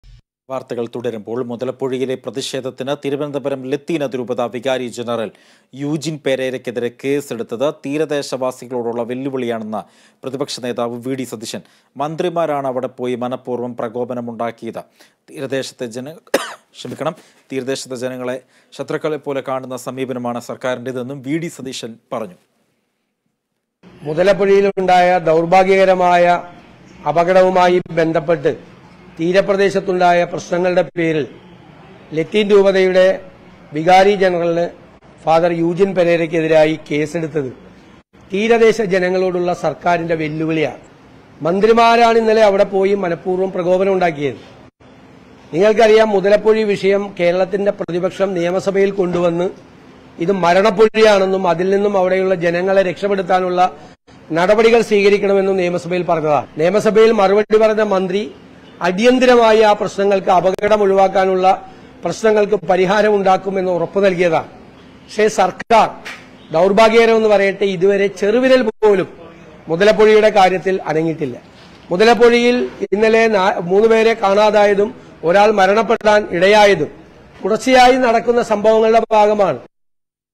യൂജിന്‍ പെരെരയ്ക്കെതിരെ കേസെടുത്തത് തീരദേശവാസികളോടുള്ള വെല്ലുവിളിയെന്ന് വി.ഡി സതീശന്‍
മുതലപ്പൊഴിയിലെ പ്രതിഷേധത്തില്‍ തിരുവനന്തപുരം ലത്തീന്‍ അതിരൂപത വികാരി ജനറല്‍ യൂജിന്‍ പേരരയ്ക്കെതിരെ കേസെടുത്തത് തീരദേശ വാസികളോടുള്ള വെല്ലുവിളിയെന്ന് പ്രതിപക്ഷ നേതാവ് വി.ഡി സതീശന്‍.
മന്ത്രിമാരാണ് അവിടെ പോയി മനപൂര്‍വ്വം പ്രകോപനം ഉണ്ടാക്കിയത്.
തീരദേശത്തെ ജനങ്ങളെ ശത്രുക്കളെ പോലെ കാണുന്ന സമീപനമാണ് സര്‍ക്കാരിന്റേതെന്നും വി.ഡി സതീശന്‍ പറഞ്ഞു.

About COA